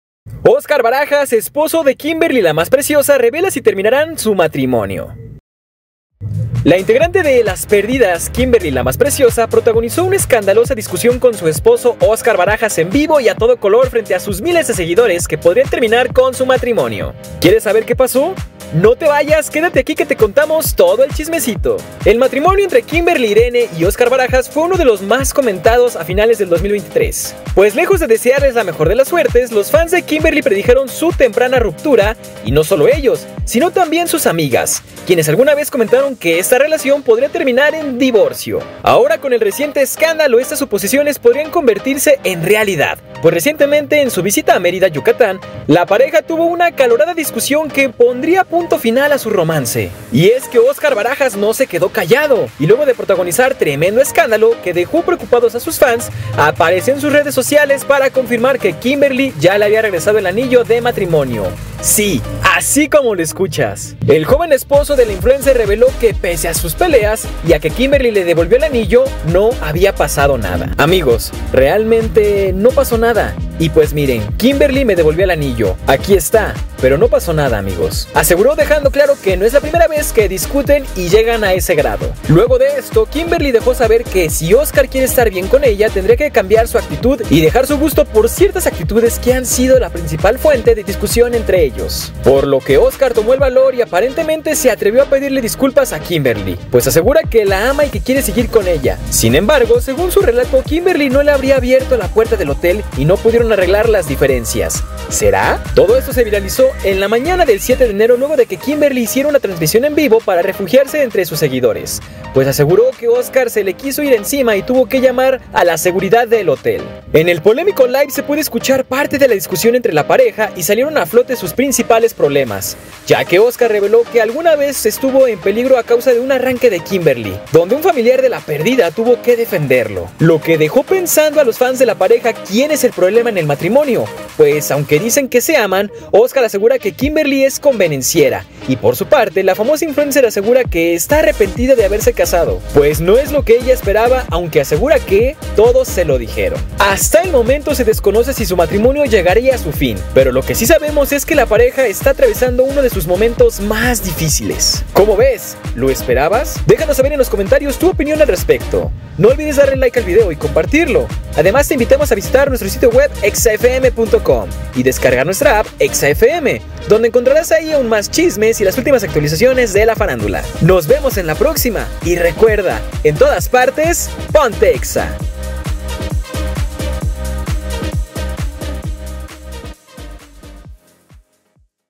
Kimberly La Más Preciosa le regresa el anillo a Óscar Barajas 😱💔💍
Kimberly La Más Preciosa, protagonizó una escandalosa discusión con su esposo Óscar Barajas en vivo y a todo color frente a sus miles de seguidores que podría terminar con su matrimonio. 💔😱💸